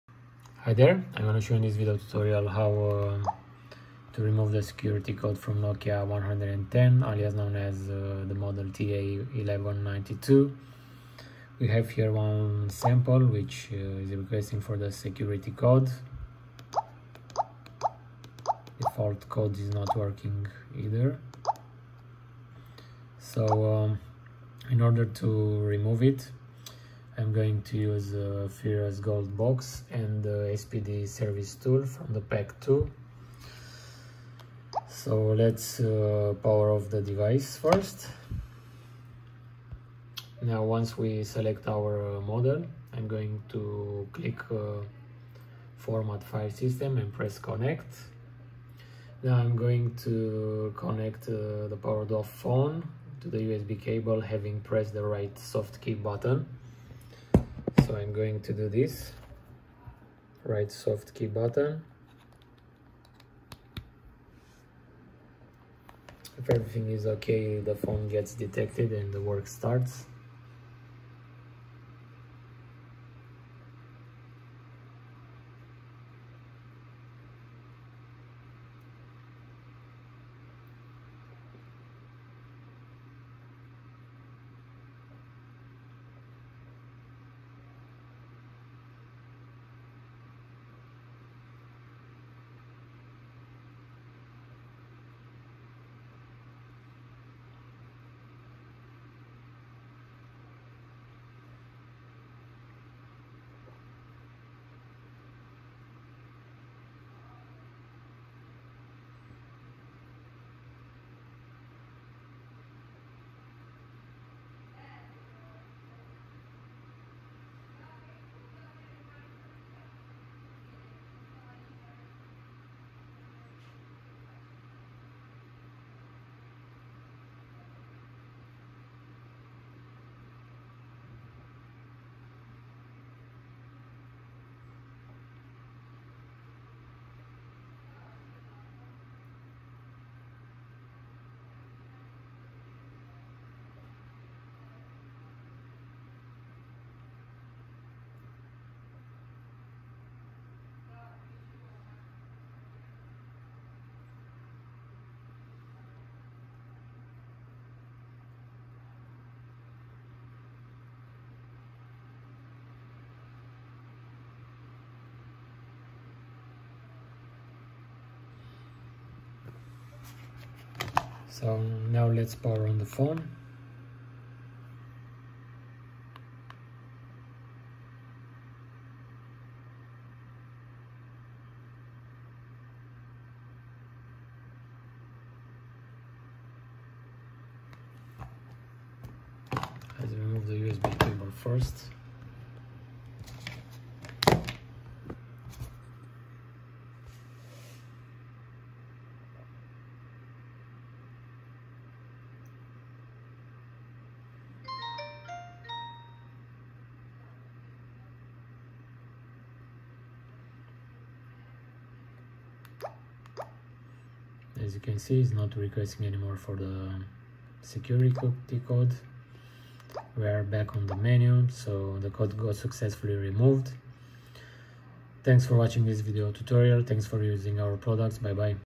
NOKIA 110 (2019) / TA-1192 / TA-1319 SECURITY CODE REMOVE USING FURIOUSGOLD
SERVICE MENU CODES: (DIAL CODE AND PRESS CALL KEY)

*#7370# - ERASE AND RESTORE FACTORY SETTINGS
8888# - SIMLOCK INFO
PW+12345678+1# - CHECK SIMLOCK LEVEL 1 (NCK)